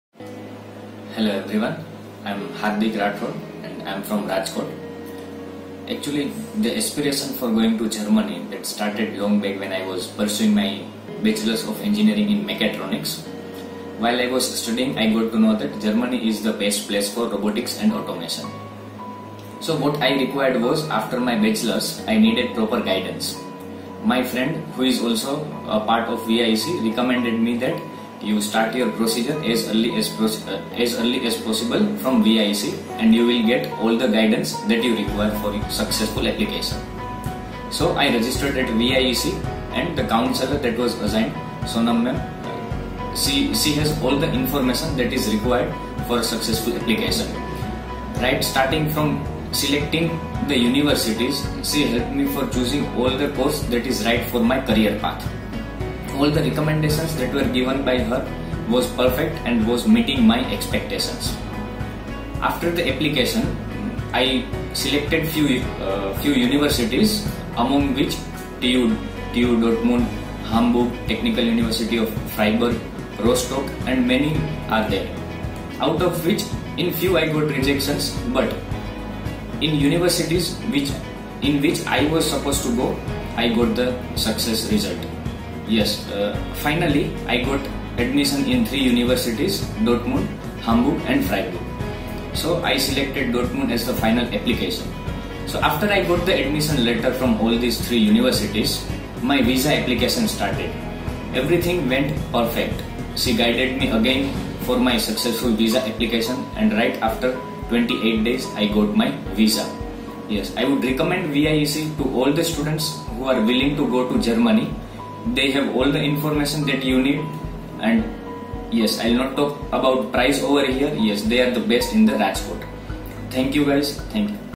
VIEC Rajkot Review by - Hardik Rathod - Germany Visa
Another feather added in pot of VIEC Rajkot. Hardik got his Germany student visa with his preferred university. Listen to him how VIEC helped him.